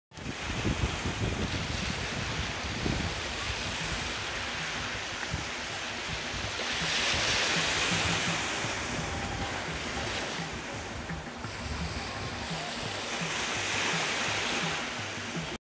🇲🇽 Mexiko / kleine Muscheln am Strand
Spezielles Lebewesen, was man selten zu sehen kriegt